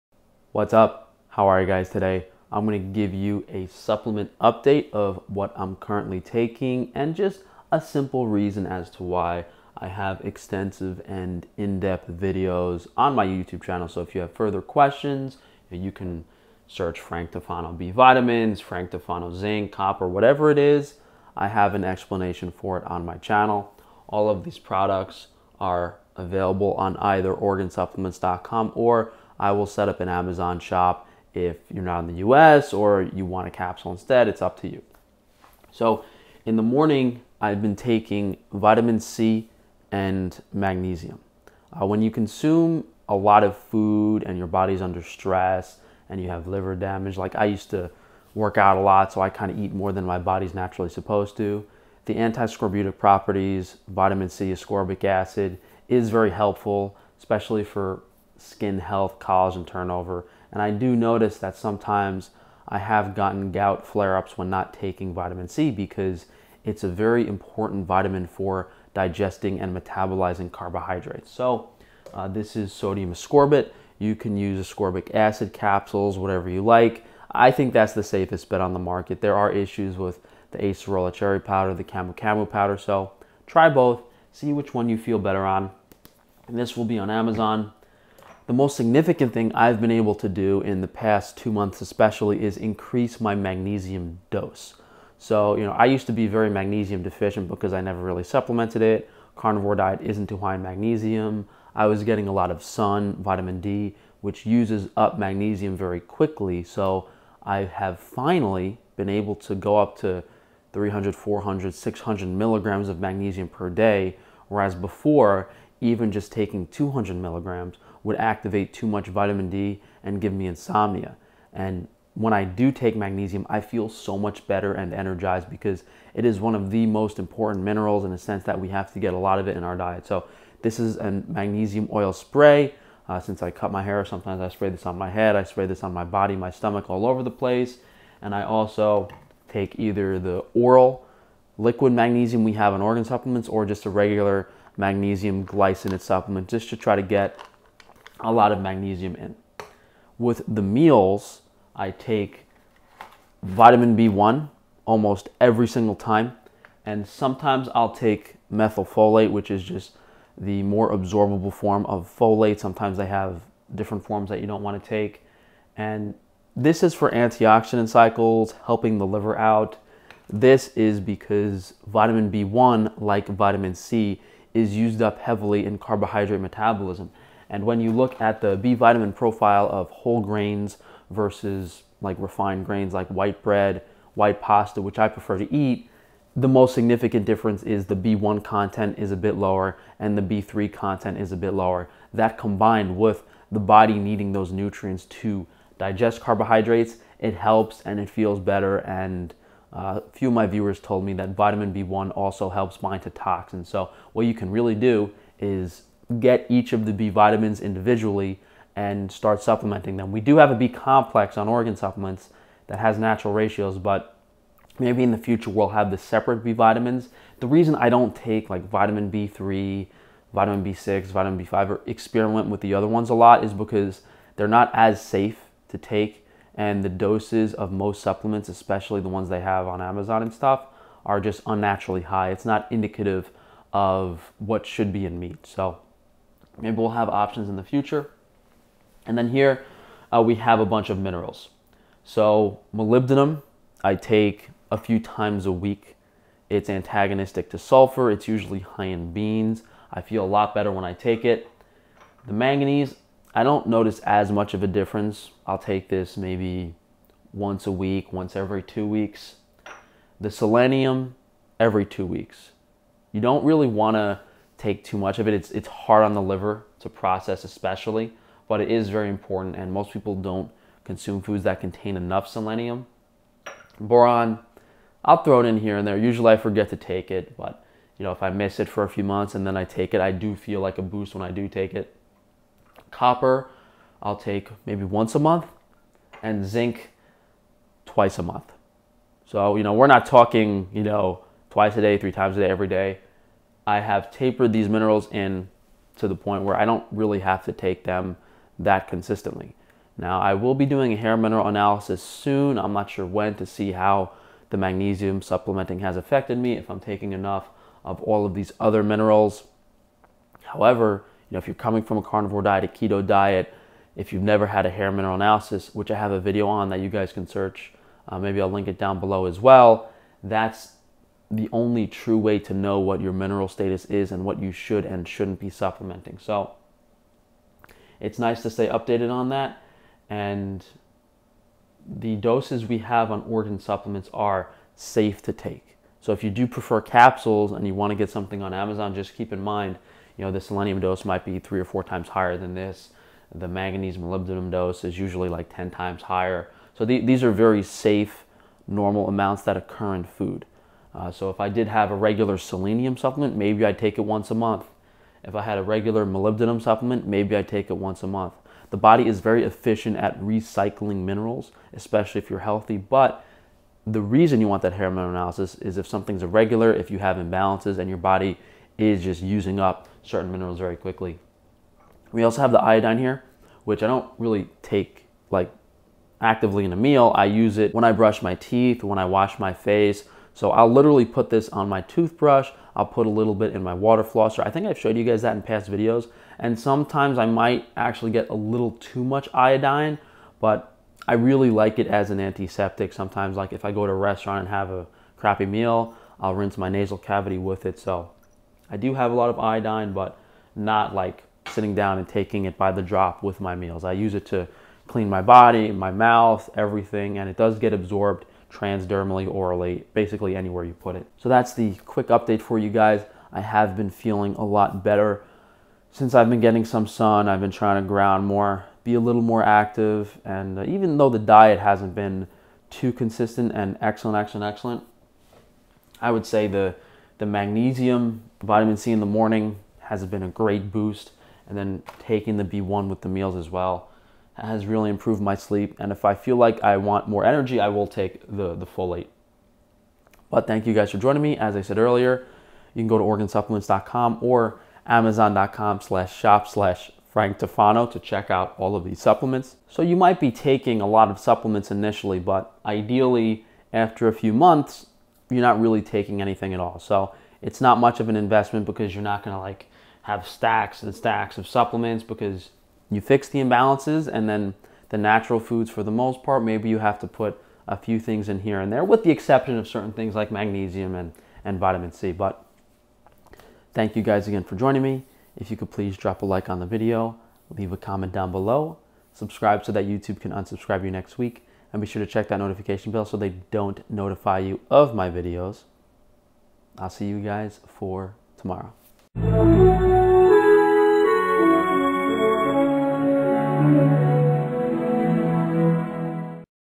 Current Supplement Routine with THE NUTRITION GENIUS DIET MASTER
00:00 - Available on organsupplements.com and
00:35 - Vitamin C and Magnesium
02:29 - Vitamin B1 and B9
03:58 - Minerals
04:59 - Hair Mineral Analysis
05:31 - Lower natural doses
06:11 - Iodine
06:50 - Feeling a bit better
07:29 - Not taking crazy amounts
07:53 - Thanks for joining